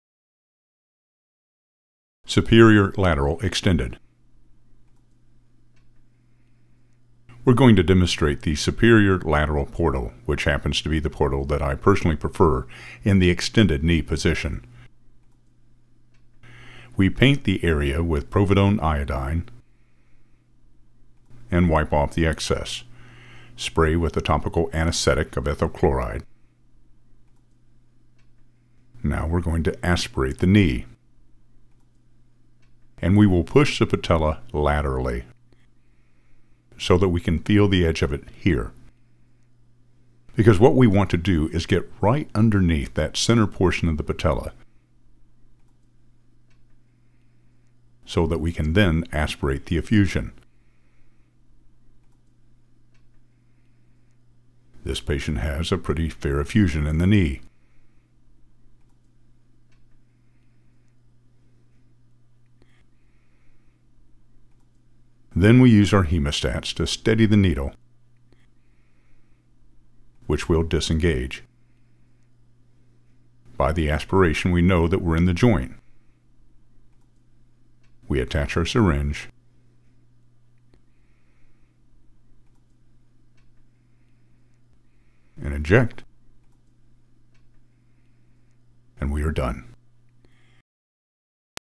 Superior Lateral Extended Knee (GELSYN-3)
Demonstration of an superior lateral extended knee injection.